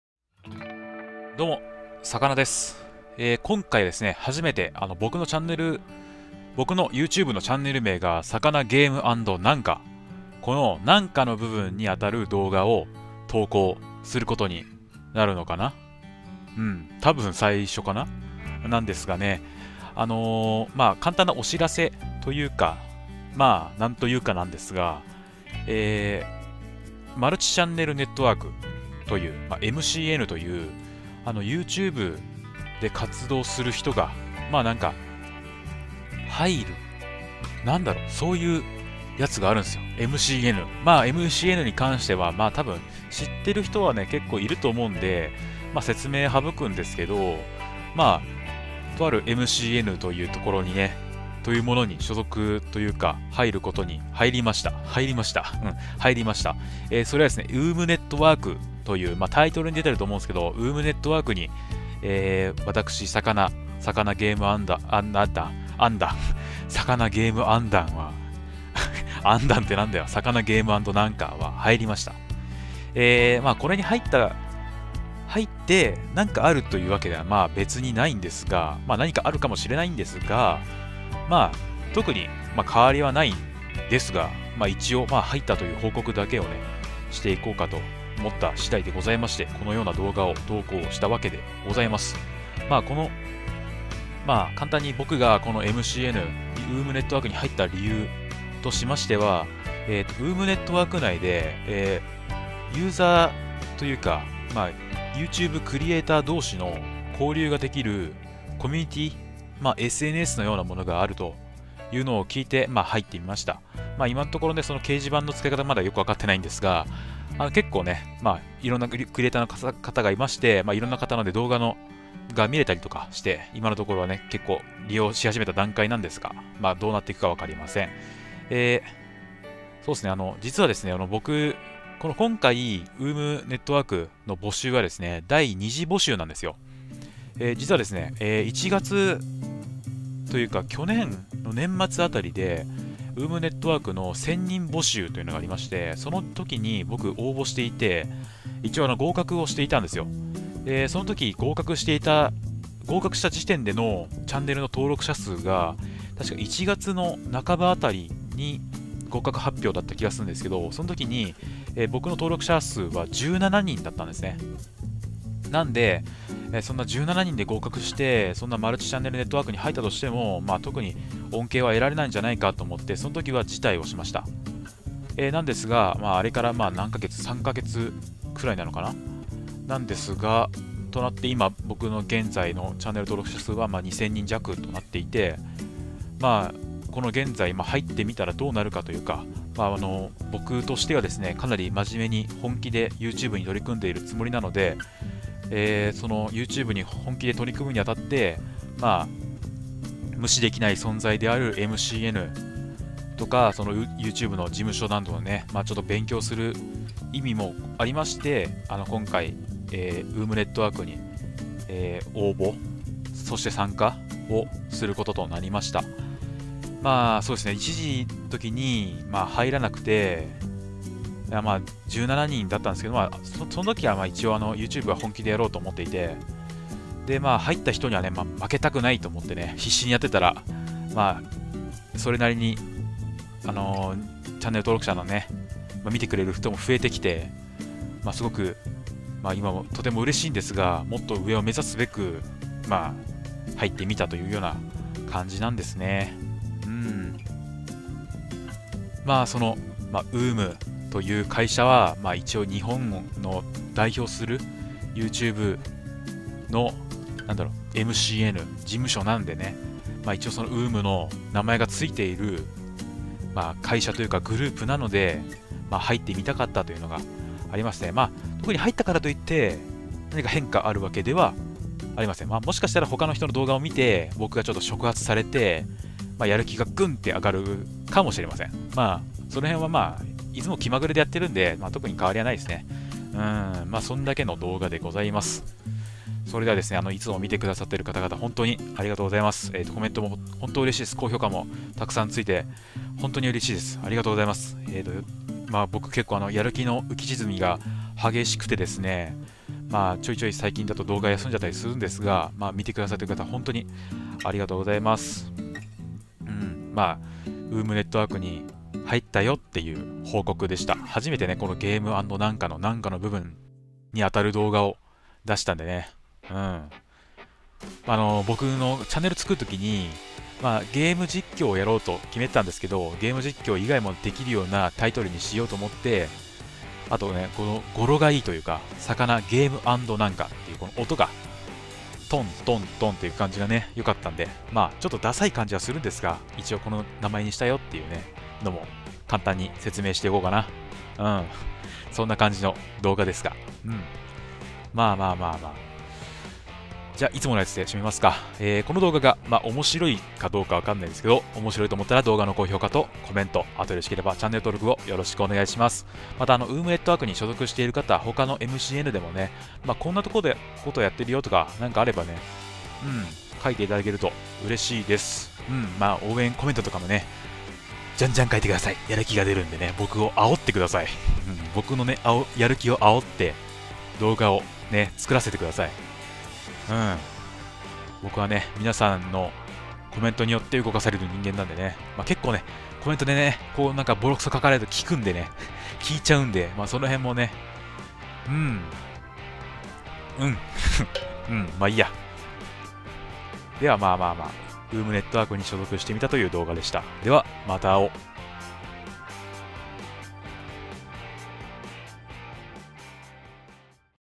UUUMネットワークに入ったよという報告 【初「＆何か」動画】
この度、UUUM NETWORK(ウームネットワーク)というMCNに加入したことを報告するべくこの動画を作りました。
ですが、加入したからといって特に動画に変化があるわけではないので見てくださってる方々には何も影響が無いと思われます。もし何かあるとすれば僕自身の変化であるため、気まぐれでやっていたこの「さかなgame＆何か」にとっては通常営業に変わりないかと思われます(笑)
なので動画にするか悩んでいたのですが、チャンネル名にゲーム実況以外もヤれるように「保険」を掛けていたので今回その保険を初適応させてみたかった！というのが本音であります。以上、ウームネットワークに入ったよの報告でした。

PSN ID→ sakanayaLugang
steam ID→sakana8desu